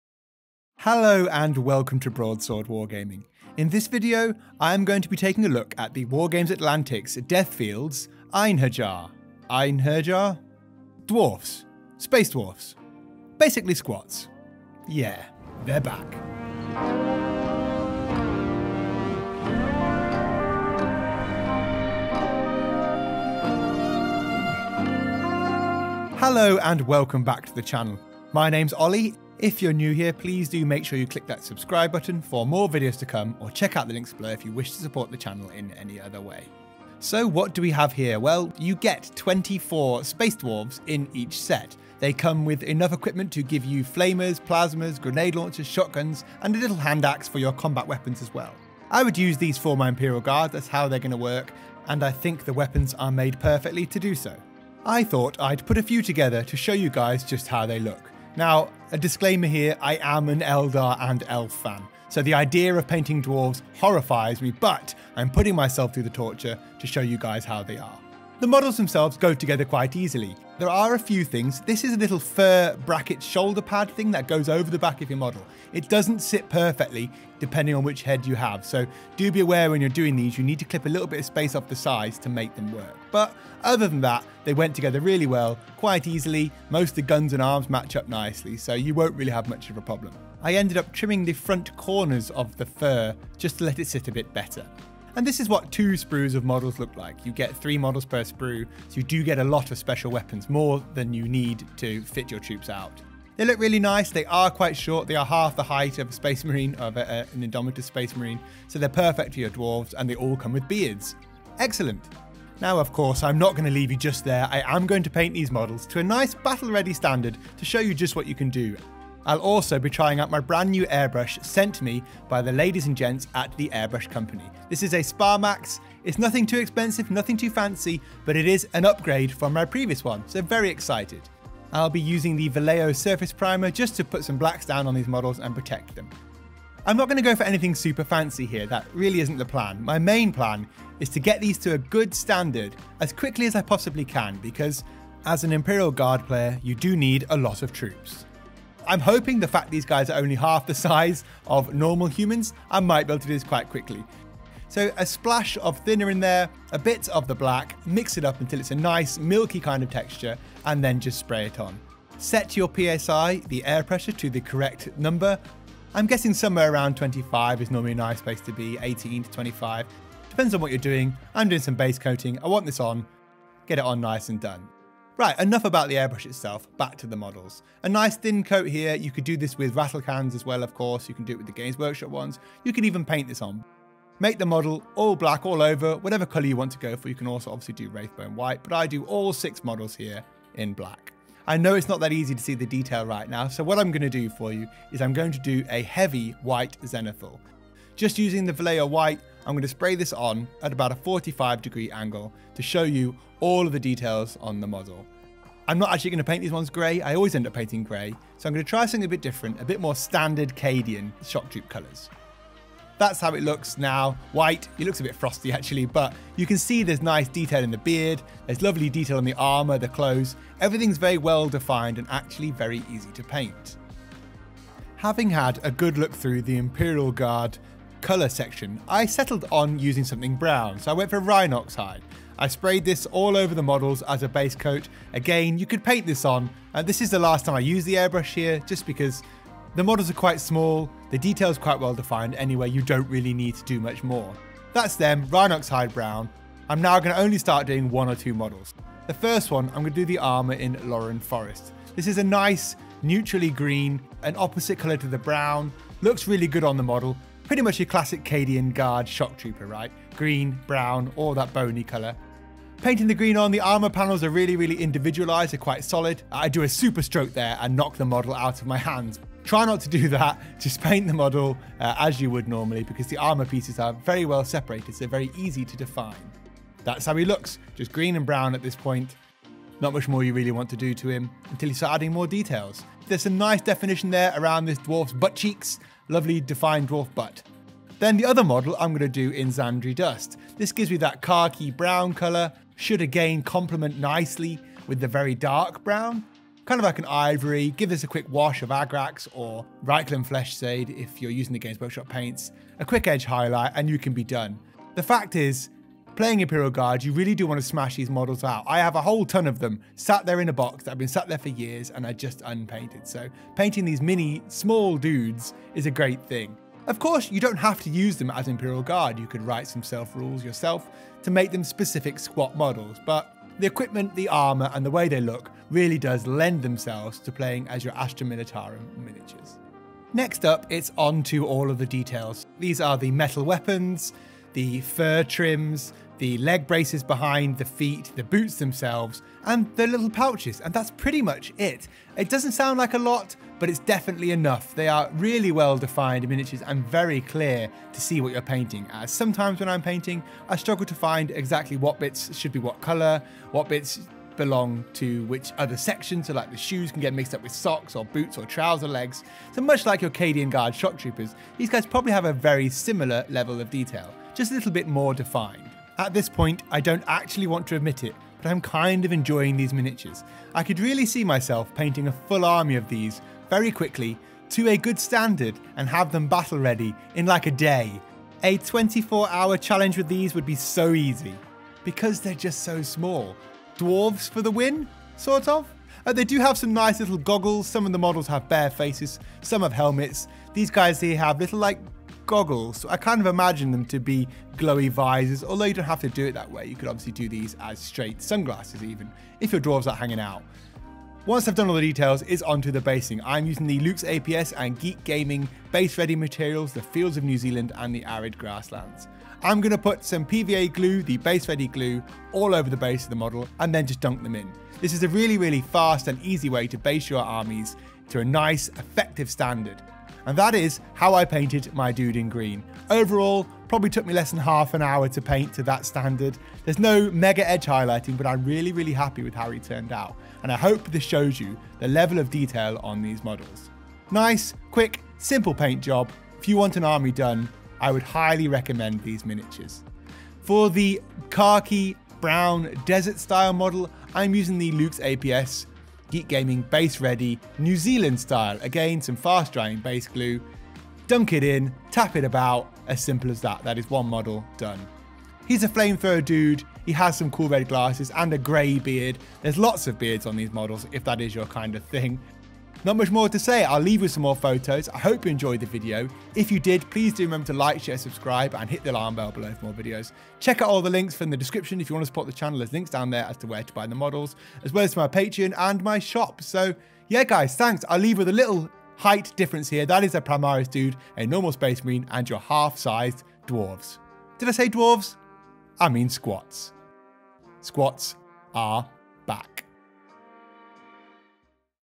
Cheap Warhammer 40k Squats.
Warhammer 40k squats. I unbox the Wargames Atlantic Space Dwarves (Einherjar) miniatures, paint them and show you how they look. Perfect for your Astra Militarum army, these work really well as Imperial Guard.

Do you enjoy the content? Your support is greatly appreciated and means a lot. 😀

Amazon shop - All your essentials